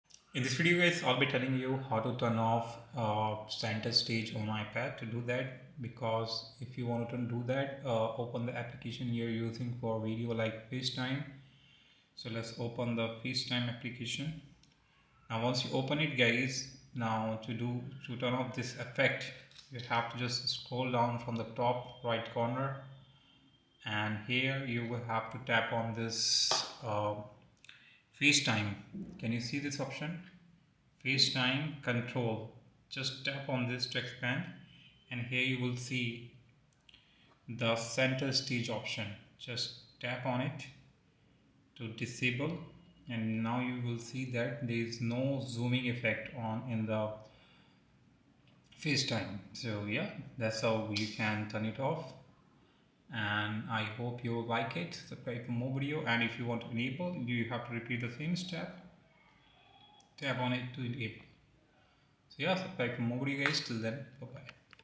How to TURN OFF Centre Stage on IPad (iOS 18 Updated Tutorial)
Are Wondering  how to turn off centre Center Stage on an IPad? This quick and easy tutorial will show you exactly how to disable the Center Stage feature on your iPad.

In this video, you'll learn:
What Center Stage is and which iPads have it.
The step-by-step process to disable Center Stage.
How to turn Center Stage back on if you change your mind.